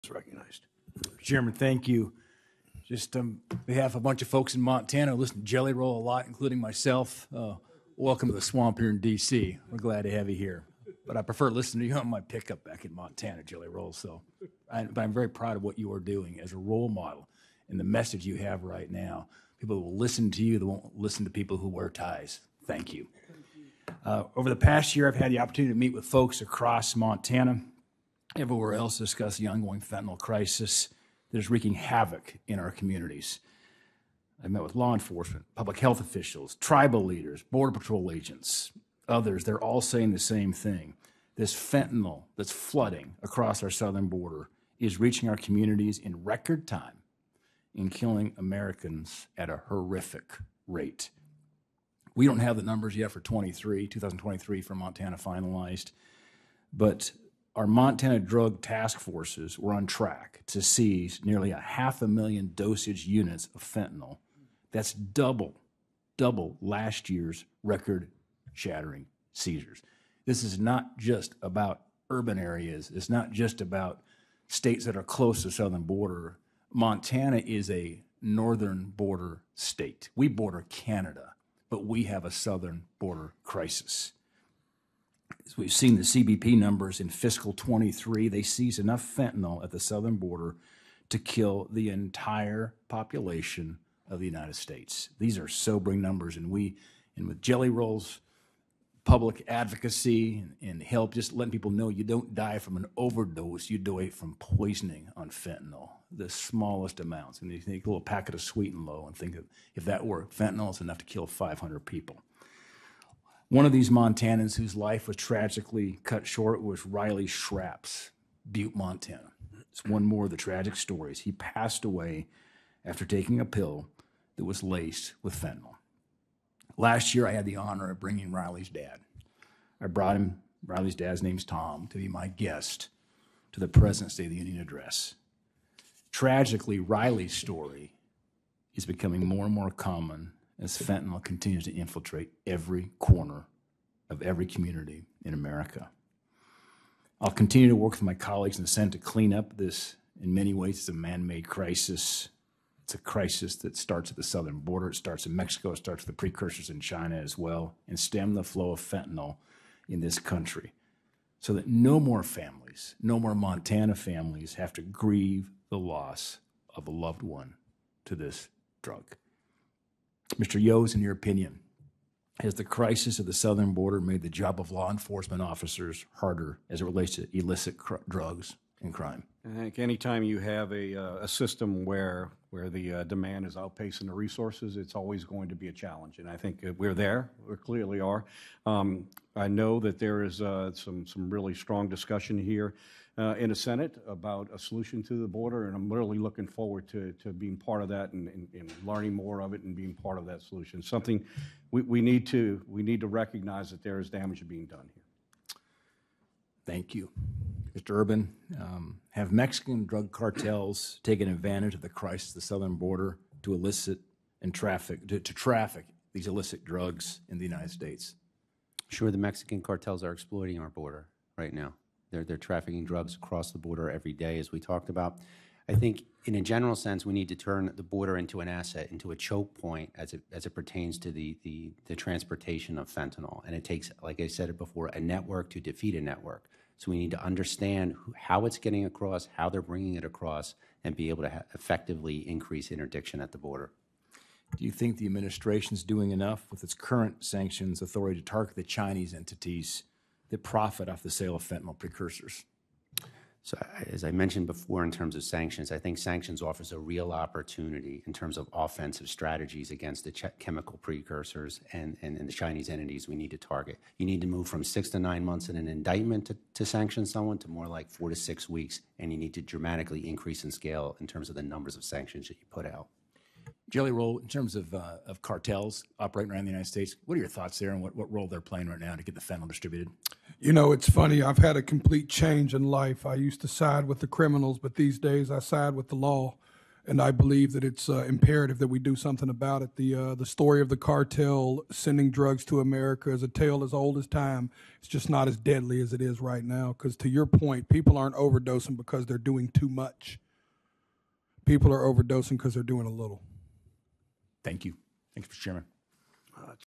"Cartel Sending DRUGS" Senator Daines and Jelly Roll in Senate Banking
During a recent discussion focused on the pressing issue of the fentanyl crisis, various individuals, including a recognized chairman, highlighted the devastating impact of this drug across the United States, particularly in Montana. The conversation involved participants from diverse backgrounds, including law enforcement, public health officials, tribal leaders, and border patrol agents, underscoring the widespread concern over the fentanyl epidemic.

A key point raised was the alarming rate at which fentanyl is infiltrating communities, not just in urban areas or states close to the southern border but also in northern border states like Montana. The crisis has been exacerbated by the substantial increase in fentanyl seizures, doubling the previous year's records. The chairman pointed out the tragic story of Riley, a victim of fentanyl poisoning, emphasizing the personal toll of the crisis.

The discussion also touched on the broader aspects of the crisis, including its origins and the role of Mexican drug cartels in exacerbating the situation. There was a consensus on the need for a multi-faceted approach, including strengthening border security, enhancing law enforcement capabilities, and targeting the supply chain of fentanyl, particularly the Chinese entities supplying precursor chemicals.

Jelly Roll, a public figure and former criminal turned law advocate, provided a unique perspective, stressing the need for effective action against the cartels and highlighting the shift in the nature of drug-related deaths - from overdose to poisoning due to the potency of fentanyl.

Overall, the conversation was a sobering reminder of the urgent need for coordinated action at both the domestic and international levels to address the fentanyl crisis effectively.